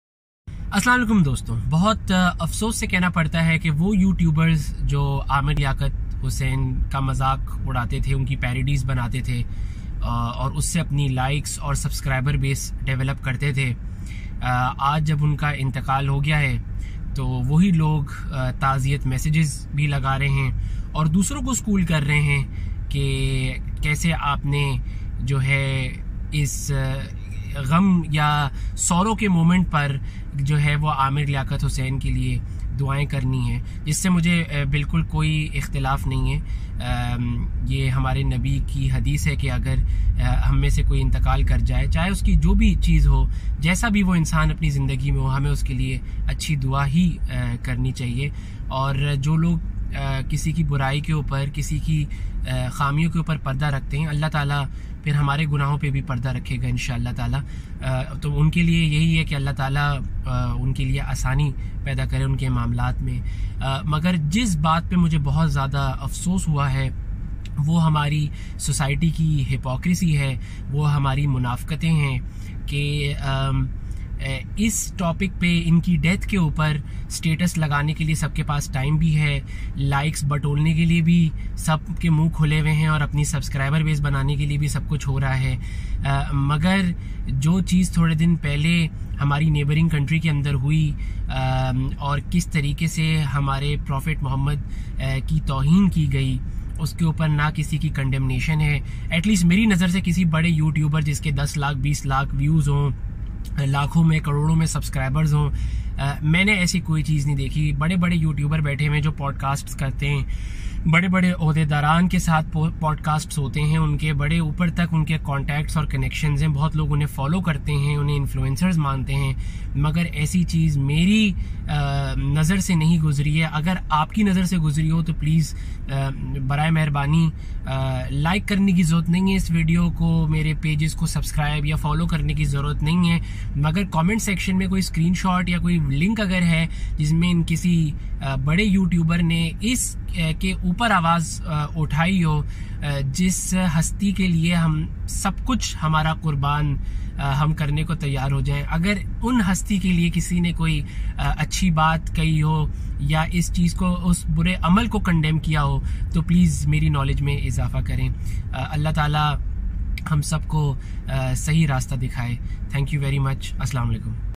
Dr. Aamer Liaquat kee death and the hypocrisy of our nation
It is our habit to make heroes out of our dead!